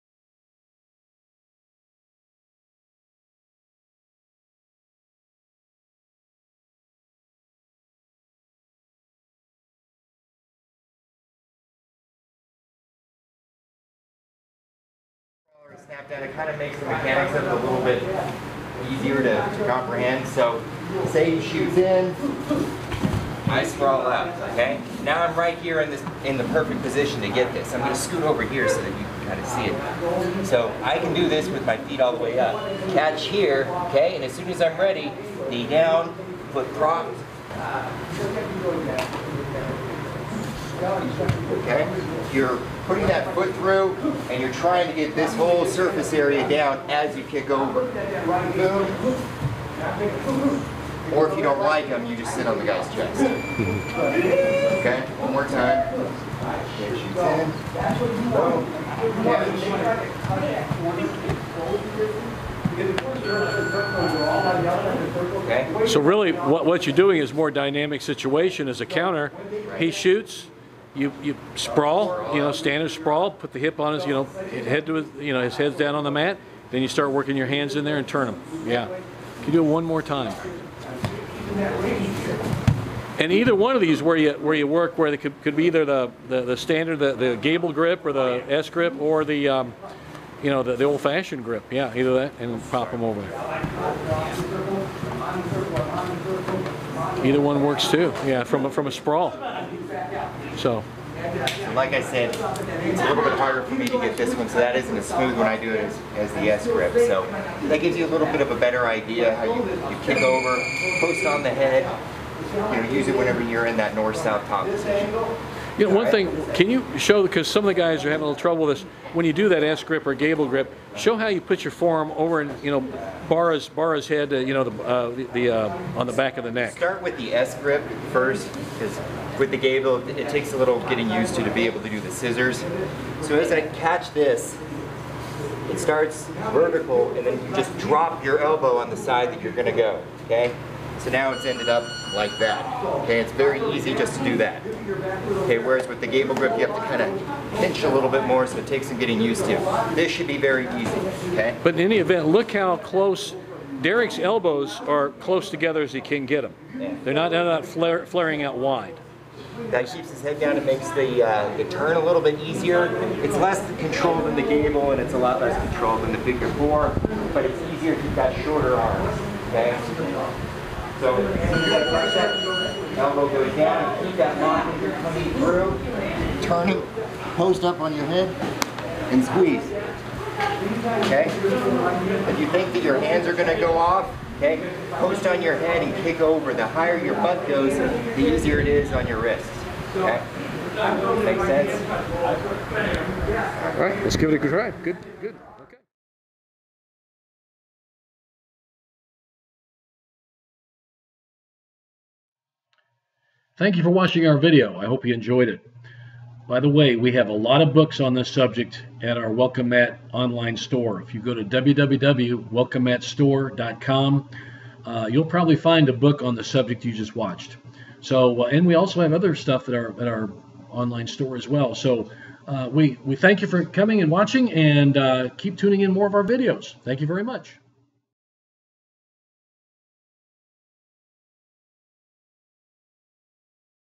GUILLOTINE CHOKE Glahn Special From Sprawl
The Guillotine Choke is a variation of Hadaka Jime (Naked Choke) and this application from a sprawl against an opponent who attacks you low. Derrick Darling coaches with Mike Pennington.